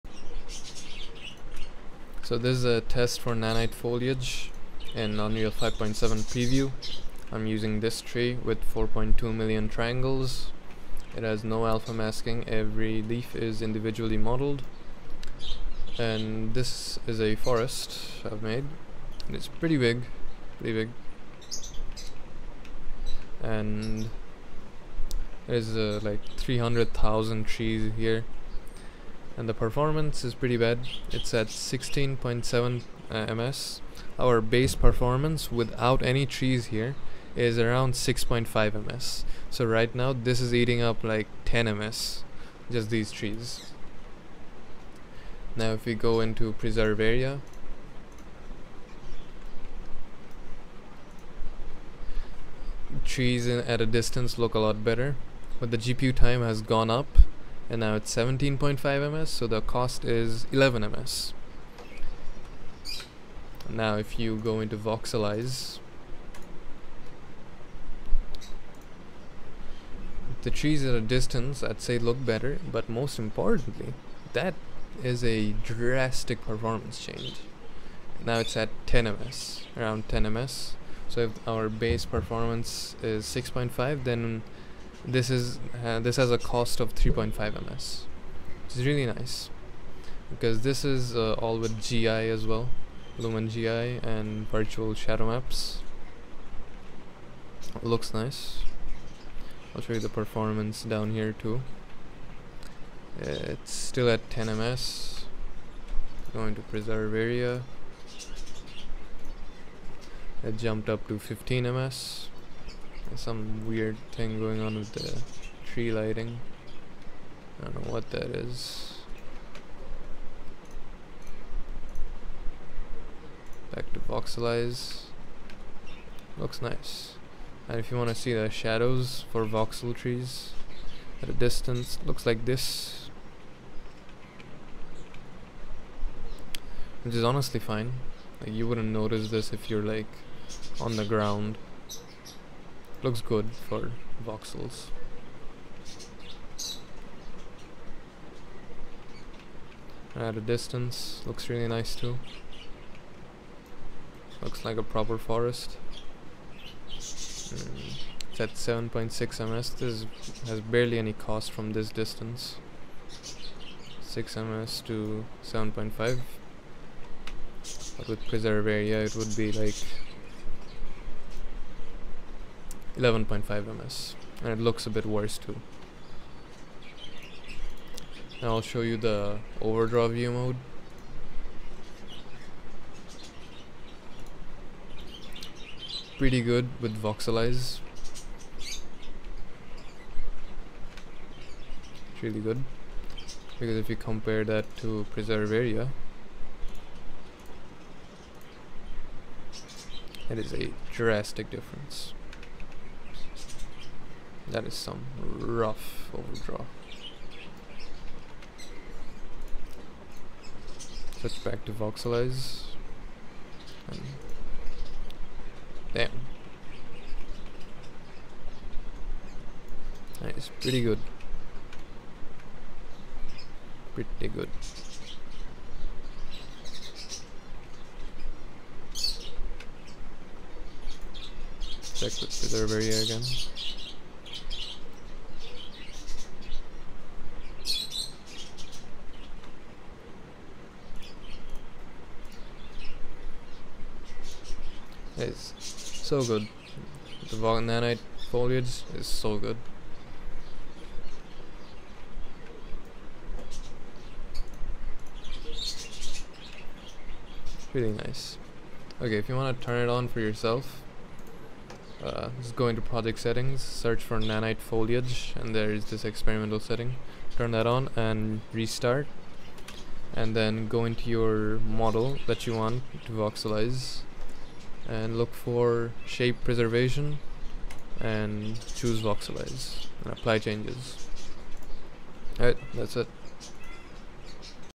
Nanite Voxel Foliage Performance and Overview + Tutorial
Nanite Foliage Performance test on unreal 5.7 preview
In this scene the voxelized foliage is performing around 3x better compared to using "preserve area". The voxelized Foliage scales very nicely, the larger the scene the greater the performance difference between "preserve area" and "voxelize"
Intro: 0:00
Performance Testing: 0:30
Nanite Foliage Tutorial: 4:15